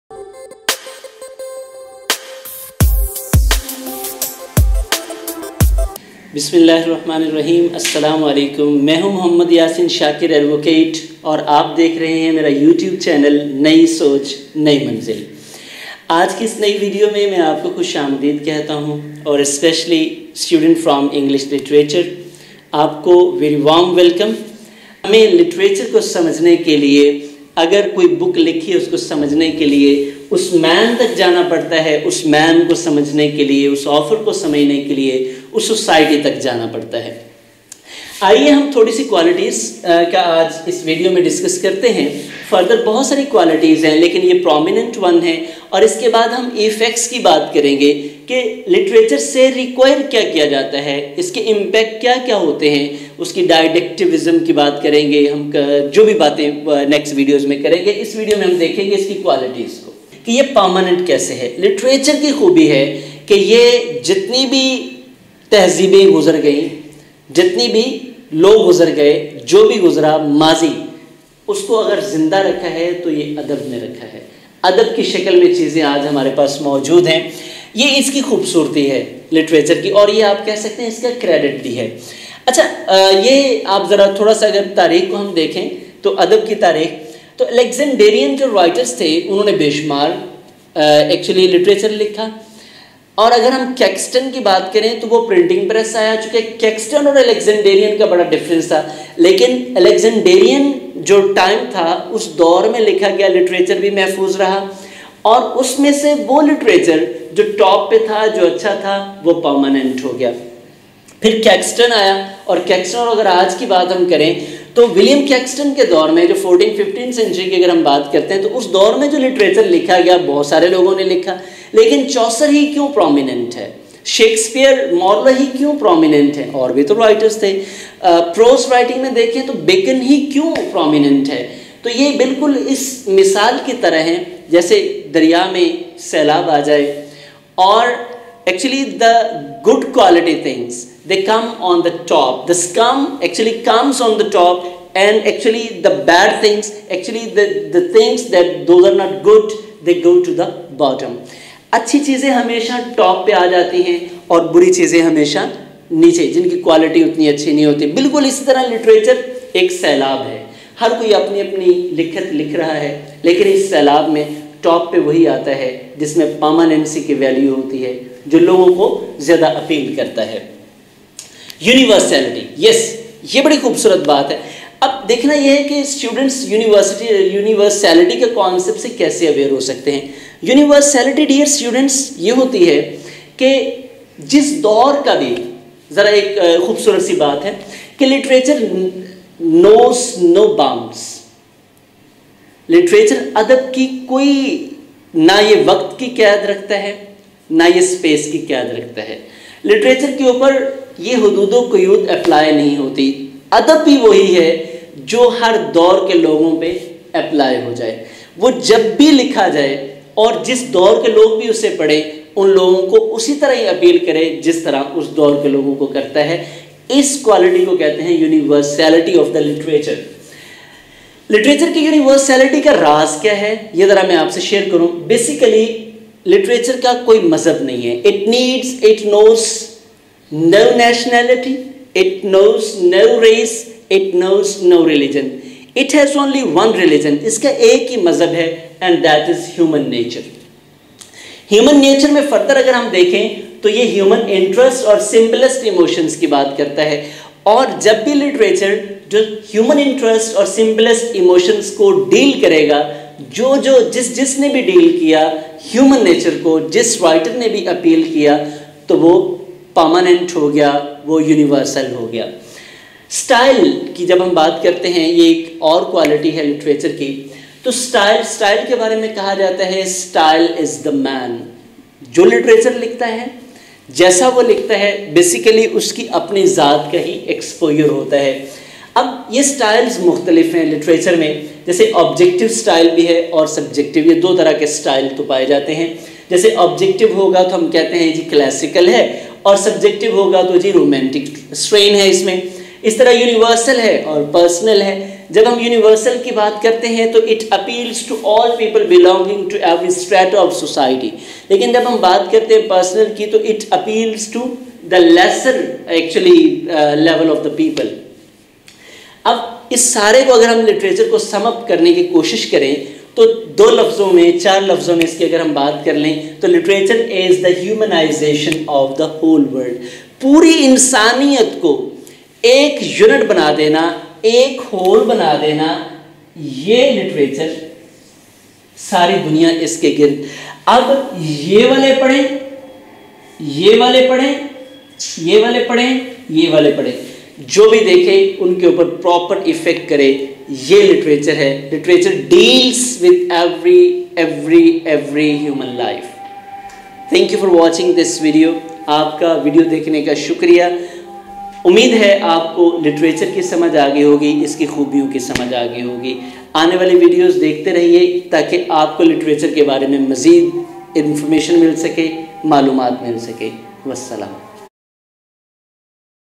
What Is Literature?Its Permanent Quality by Yasin Shakir Adv for B.A, M.A Eng, UGC,NET in Urdu/Hindi
Dear viewers, hope you would be hale and hearty! Students from English literature! thumbs up! be ready! Here is mr .Yasin Shakir Adv discussing human life : i mean LITERATURE and its permanent&UNIVERSAL qualities.. What is literature? and how can we understand it in a better way?It is stepping stone for the beginners and refinement for the furnishing. Please have it and enjoy the beauty of your aestheticism...